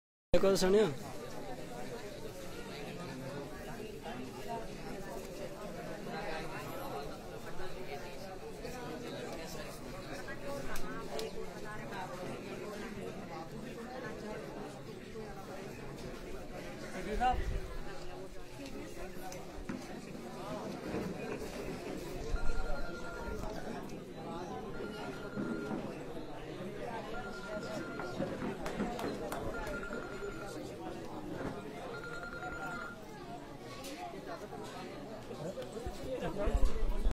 आज निवाई के चैनपुरा पंचायत के ग्राम भावता में अतिरिक्त जिला कलेक्टर साहब की अध्यक्षता में जनसुनवाई
आज चैनपुरा पंचायत के ग्राम भावता में अतिरिक्त जिला कलेक्टर साहब की अध्यक्षता में जनसुनवाई की कार्यक्रम की झलकियां
निवेदक- चैनपुरा पंचायत सरपंच श्रीमान मदन लाल मीणा भावती
निवाई शहर महासचिव सुदामा चौधरी भांवती
समस्त चैनपुरा पंचायत वासी